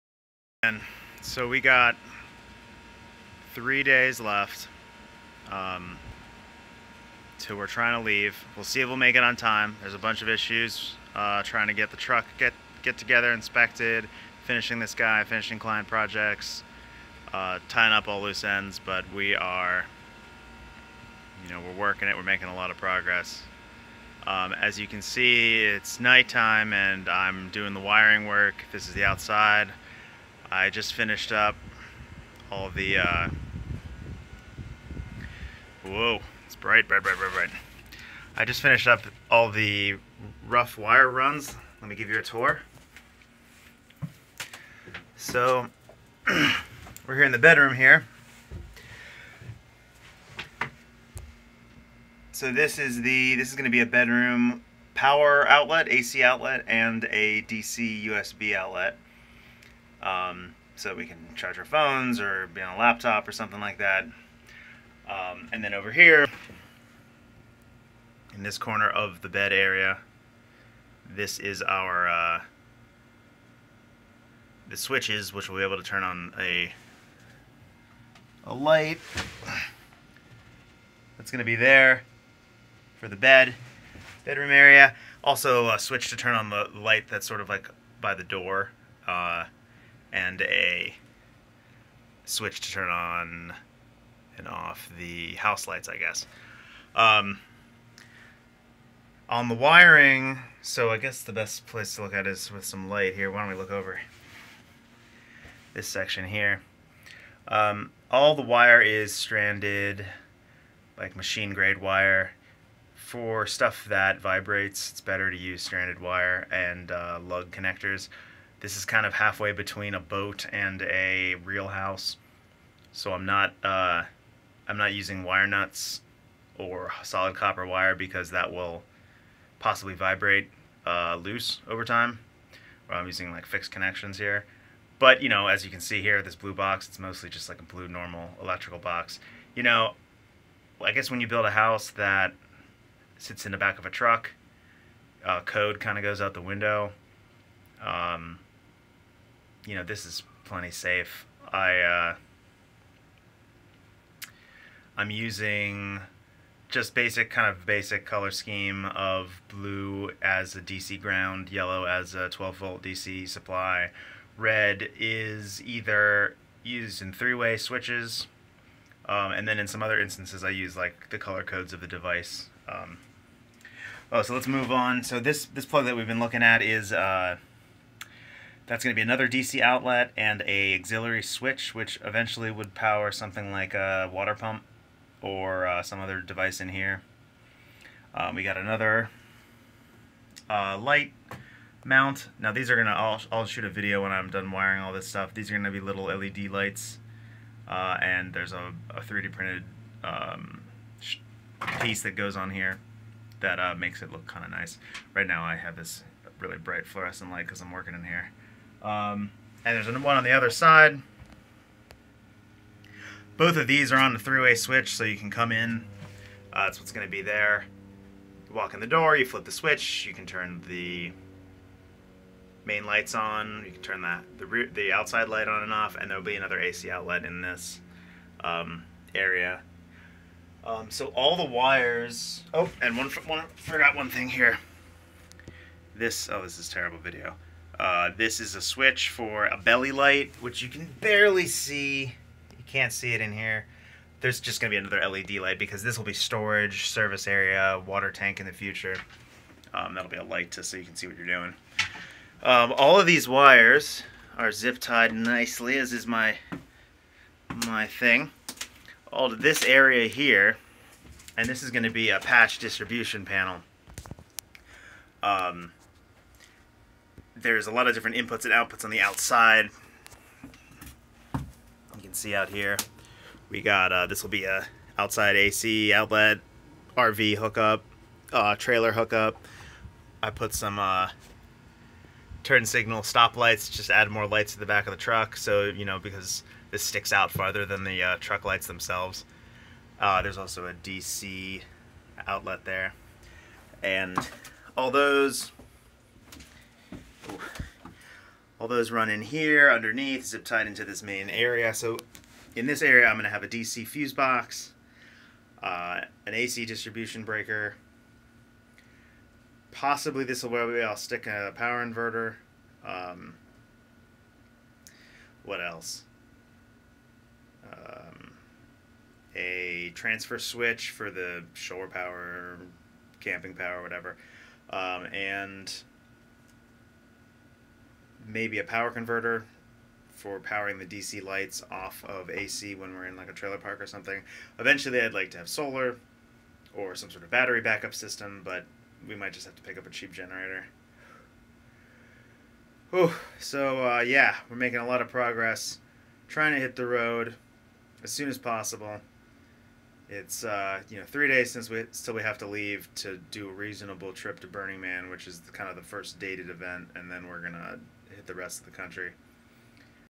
Ford Flophouse Wiring: T-3 Day
In this video I give an overview of all the wiring systems in the house